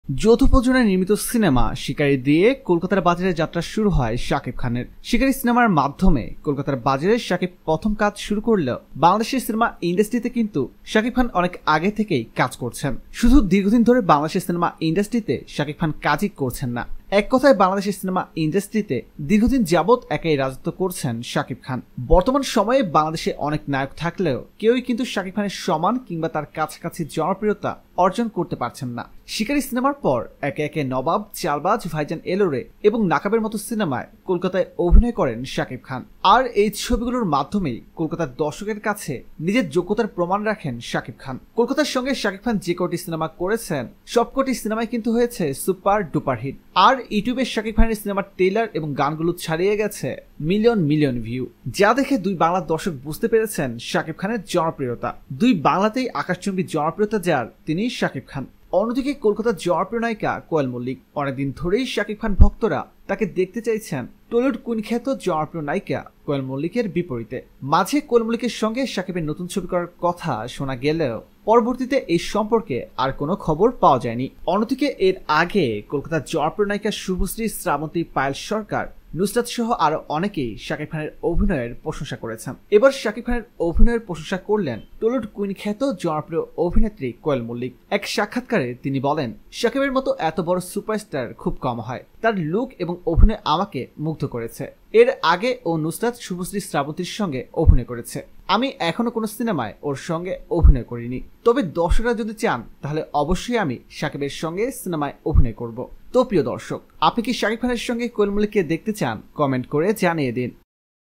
শাকিব খানের অভিনয় নিয়ে একি বললেন কোয়েল মল্লিক !! Koel Mallick with Shakib Khan
শাকিব খানের অভিনয় নিয়ে একি বললেন কোয়েল মল্লিক !! Koel Mallick with Shakib Khan | Latest Bangla News

যৌথ প্রযোজনায় নির্মিত সিনেমা শিকারি দিয়ে কলকাতার বাজারে যাত্রা শুরু হয় শাকিব খানের। শিকারি সিনেমার মাধ্যমে কলকাতার বাজারে শাকিব প্রথম কাজ শুরু করলেও, বাংলাদেশের সিনেমা ইন্ডাস্ট্রিতে কিন্তু শাকিব খান অনেক আগে থেকেই কাজ করছেন। শুধু দীর্ঘদিন ধরে বাংলাদেশের সিনেমা ইন্ডাস্ট্রিতে শাকিব খান কাজই করছেন না, এক কথায় বাংলাদেশের সিনেমা ইন্ডাস্ট্রিতে দীর্ঘদিন যাবত একাই রাজত্ব করছেন শাকিব খান।